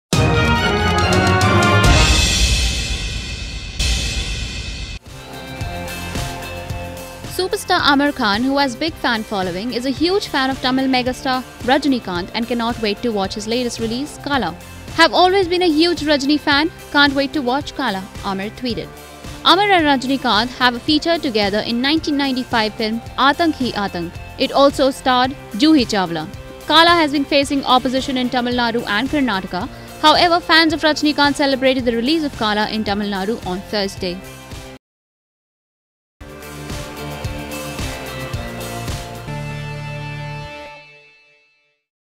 Mr Perfectionist Aamir Khan is HUGE FAN of Rajinikanth
Superstar Aamir Khan, who has big fan following, is a huge fan of Tamil megastar Rajinikanth and cannot wait to watch his latest release "Kaala".   Aamir and Rajinikanth have featured together in the 1995 film "Aatank Hi Aatank". It also stars Juhi Chaw